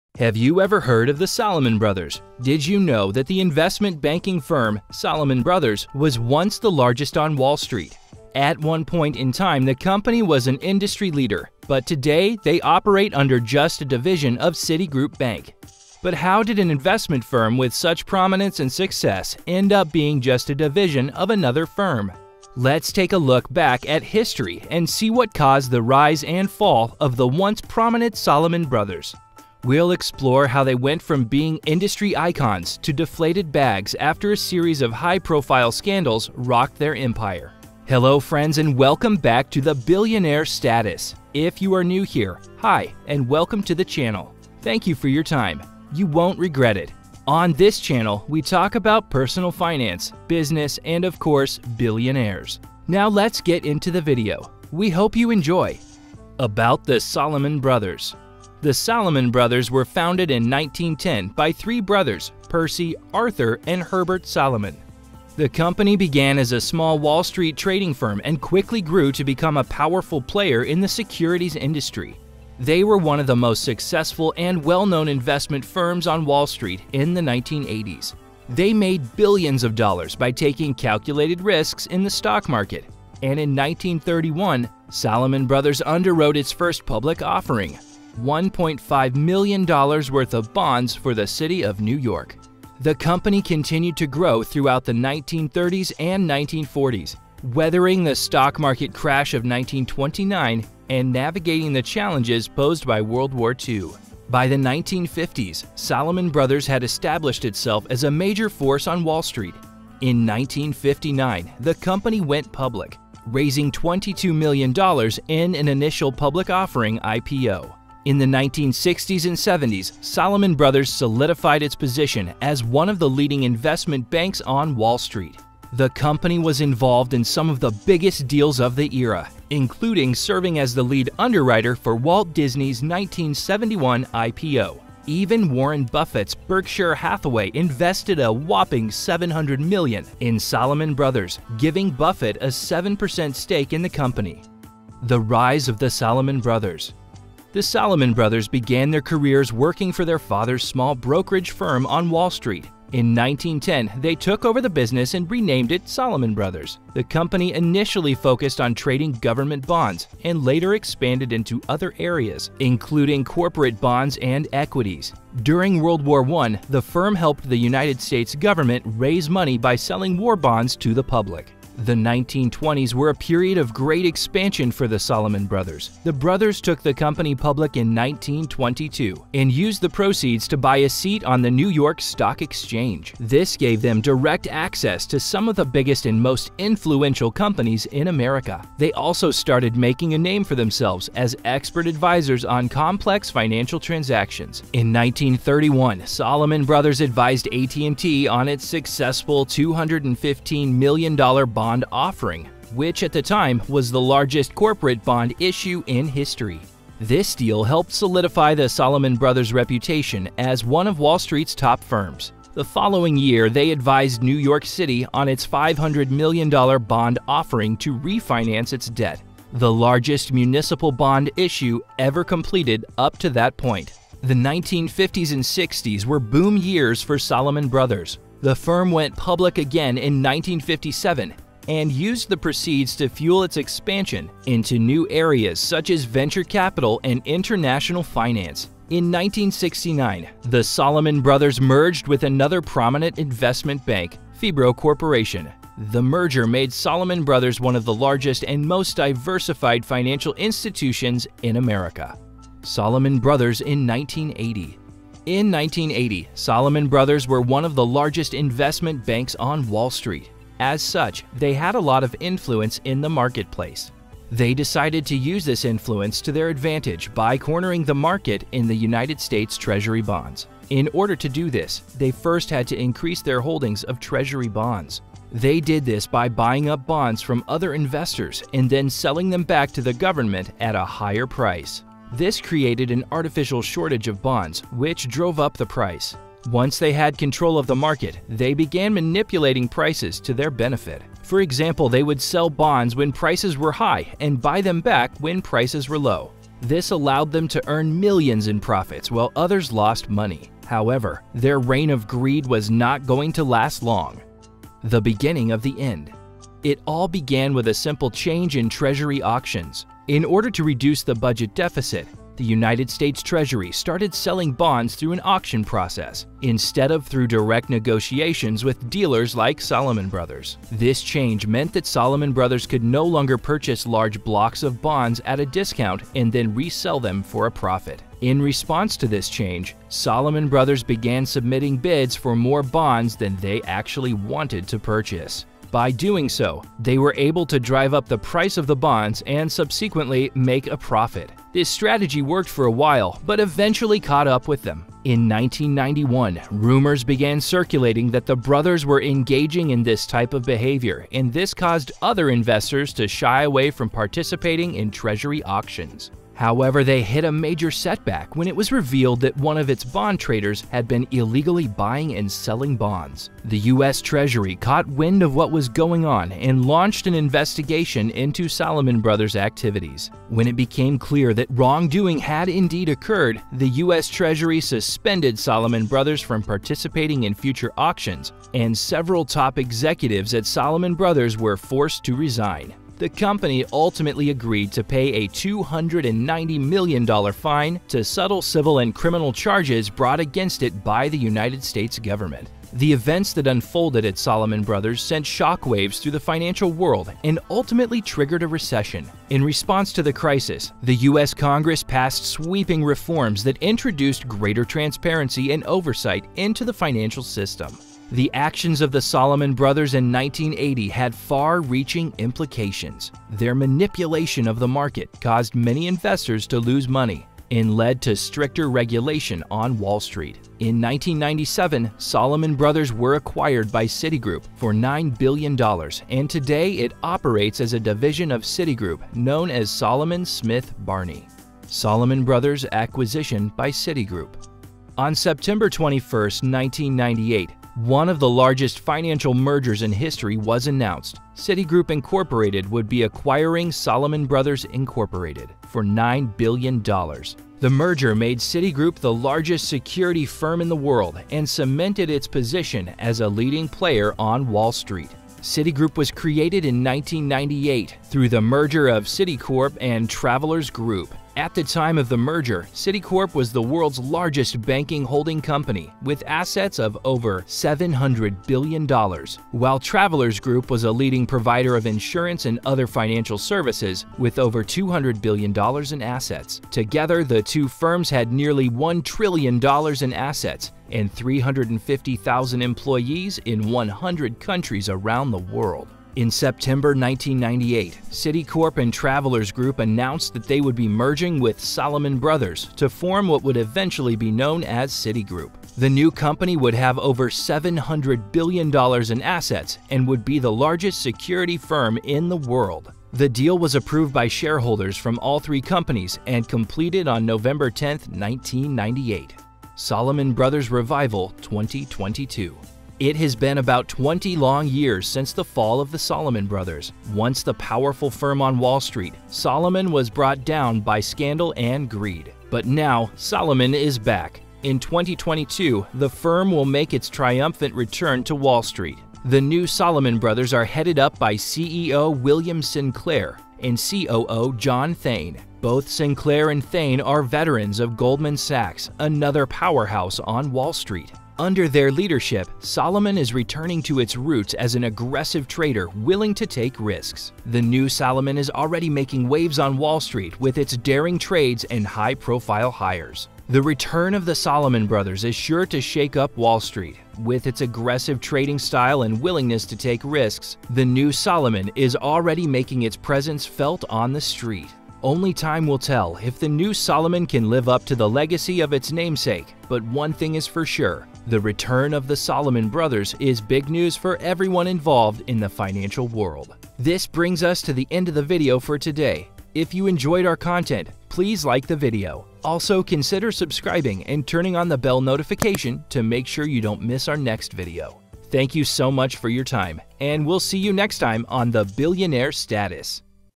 The Rise and Fall of Salomon Brothers
In this video, we'll explore the rise and fall of the Salomon Brothers, one of the most famous and successful investment banks on Wall Street.

This video is a great way to learn about Wall Street and the investment banking industry. We'll explore the history of the Salomon Brothers and see how they rose to fame and fortune. We'll also look at their fall and see how their disastrous investment decisions led to their eventual demise. This video will provide you with a comprehensive overview of the Salomon Brothers and their important role in financial history!